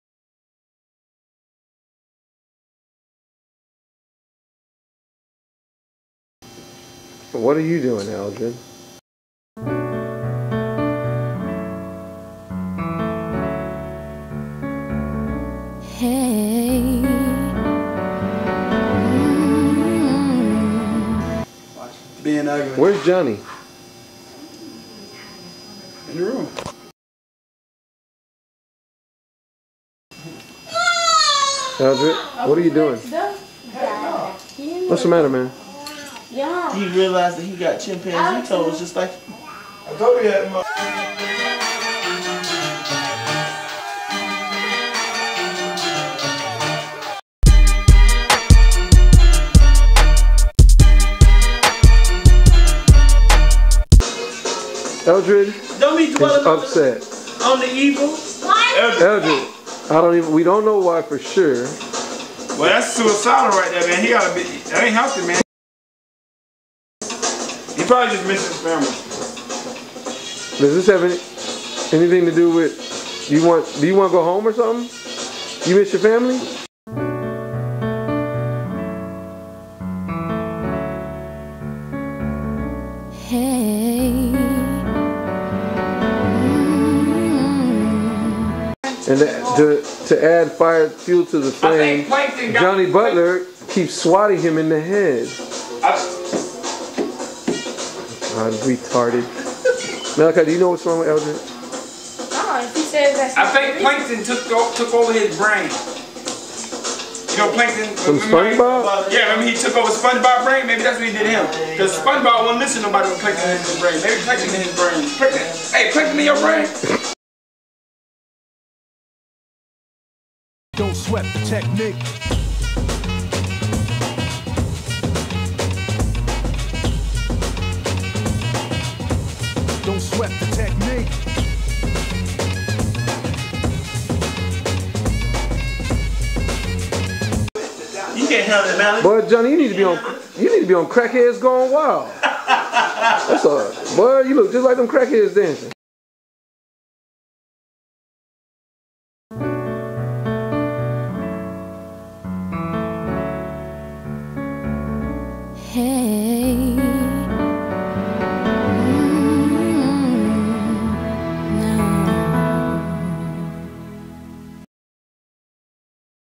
Episode 3: What's wrong with Eldred?
Eldred, our eternal houseguest, is depressed and wont say why. The kids are concerned (sort of). Also, we introduce Johnny (J'be), our new houseguest. Now we have 11 people living with us.